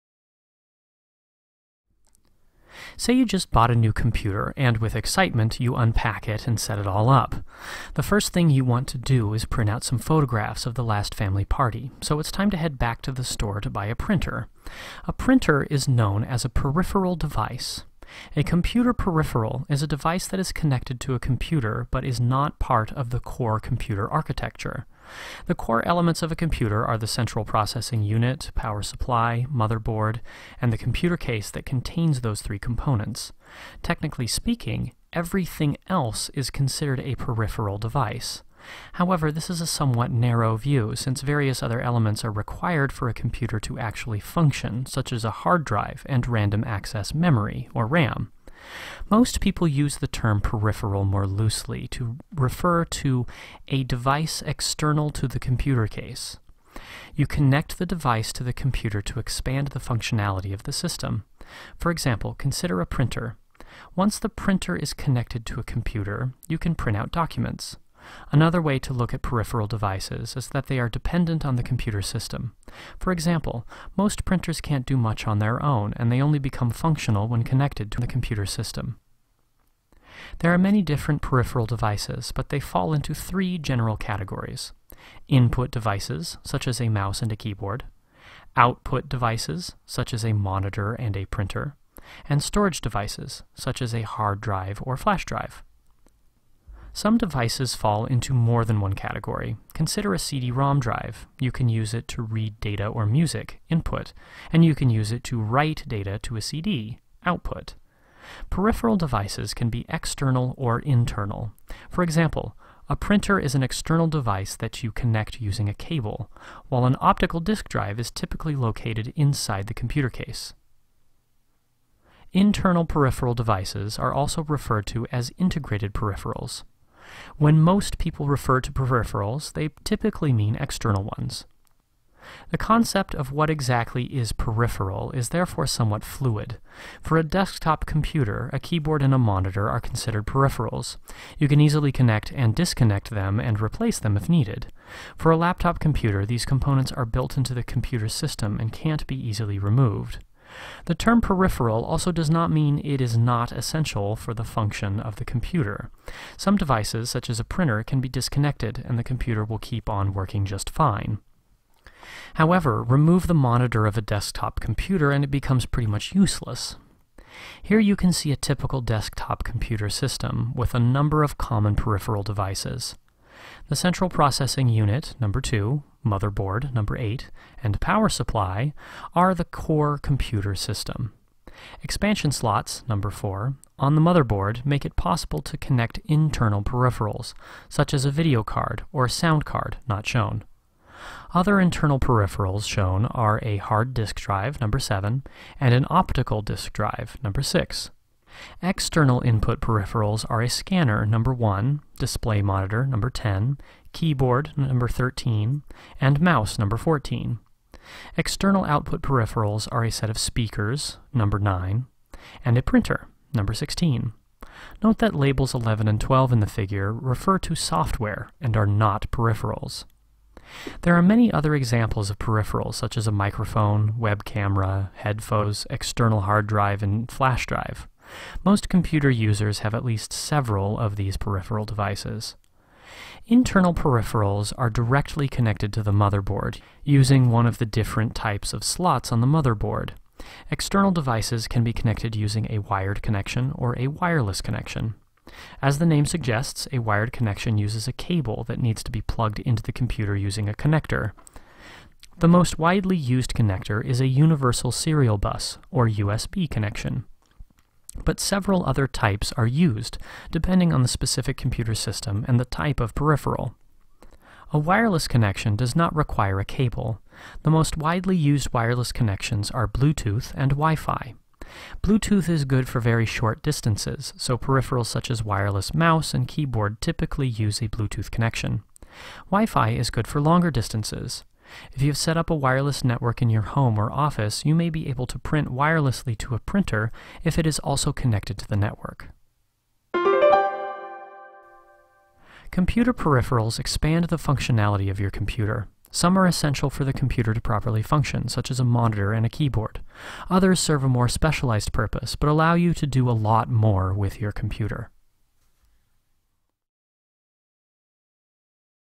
What Are Peripheral Devices of a Computer Definition, Examples & Types
This is a brief explanation on  peripheral devices.